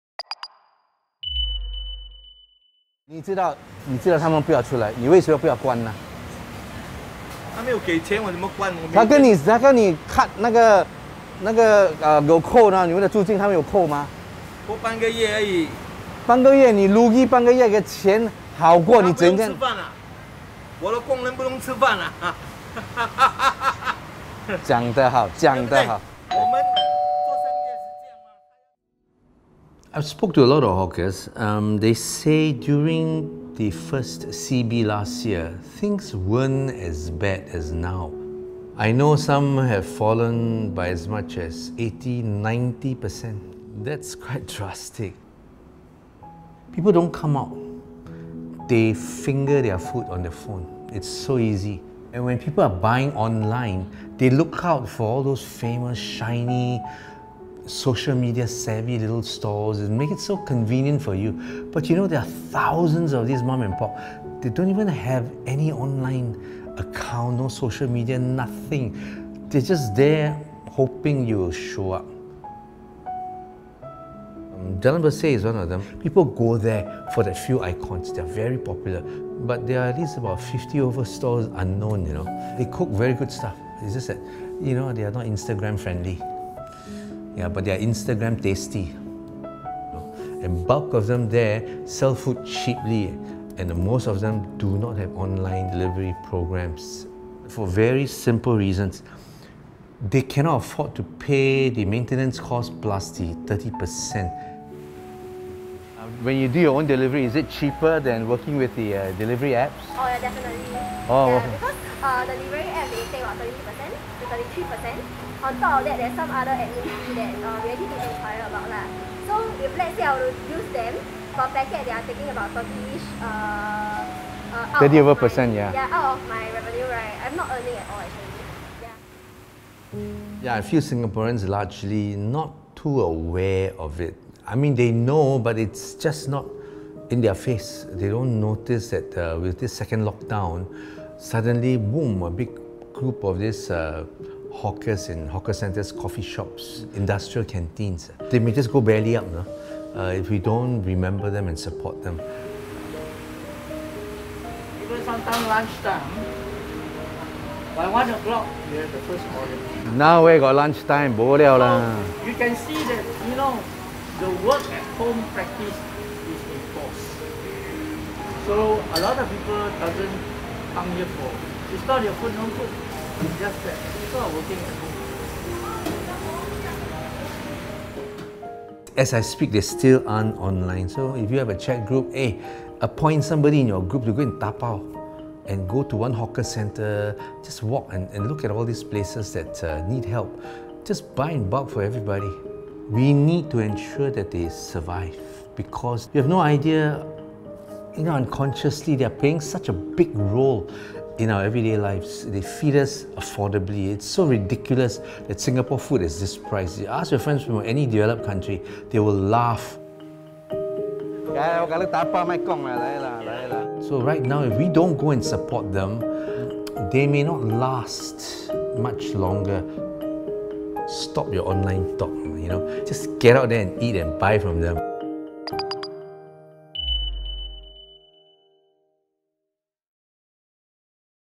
How We Can Support Our Offline Hawkers
With restaurants and eateries on social media and delivery apps at the tips of our fingers, we forget the many hawkers that slip through the cracks.

We join KF Seetoh from Makansutra on a walk around Berseh Food Centre to find these hidden gems.

📍Location Details
Berseh Food Centre
166 Jalan Besar
Singapore 208877